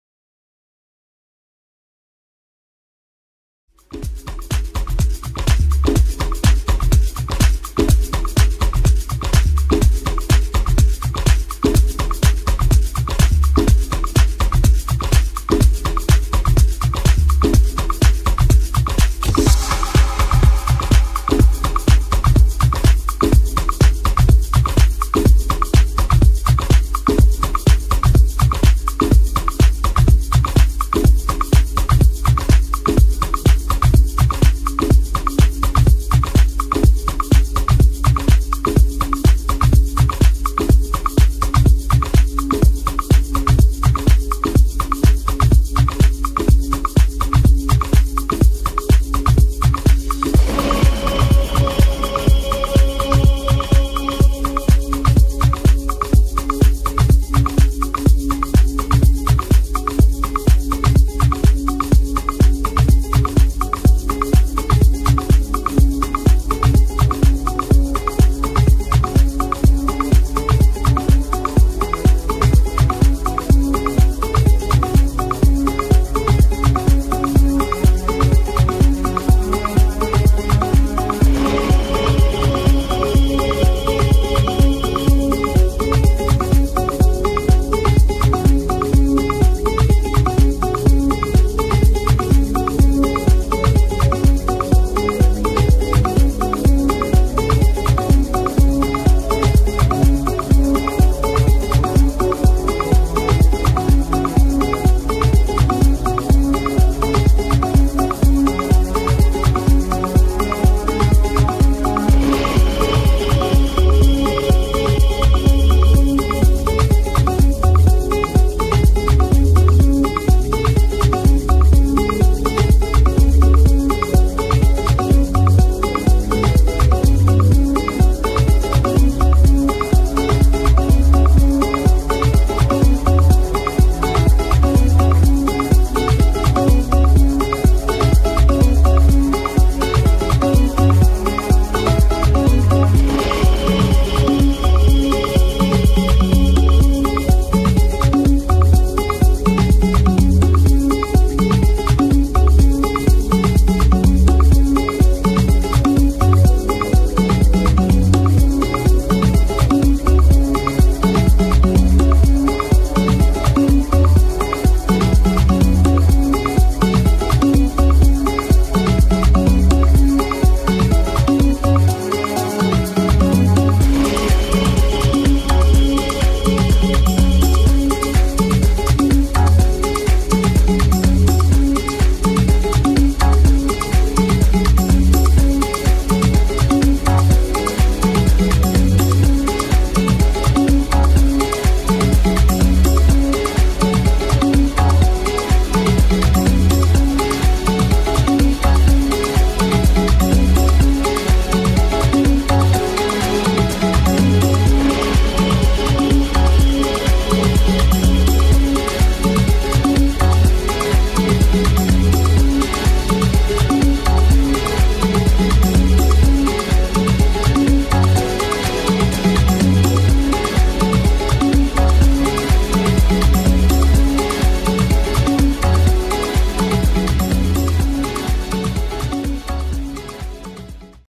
Citroen C4 WRC NOREV 1:18 Sebastien Loeb - Daniel Elena [4K]
Citroen C4 WRC scale car 1:18 Norev (with custom paintings)
Sebastien Loeb - Daniel Elena
Product code: 181550
Citroen Racing